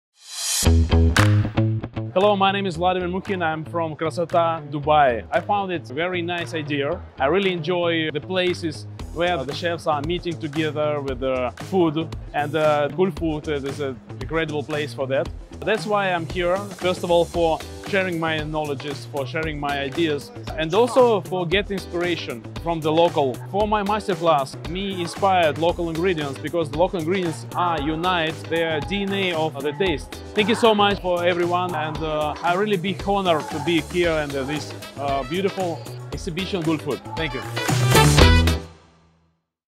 CHEF VLADIMIR MUKHIN - GULFOOD 2024
In an inspiring chat with Chef Vladimir Mukhin of Krasota Dubai, he shared his excitement about Gulfood being a melting pot where international chefs unite. He's here not just to exchange ideas and share his expertise, but also to draw inspiration from the vibrant local food scene. 🍴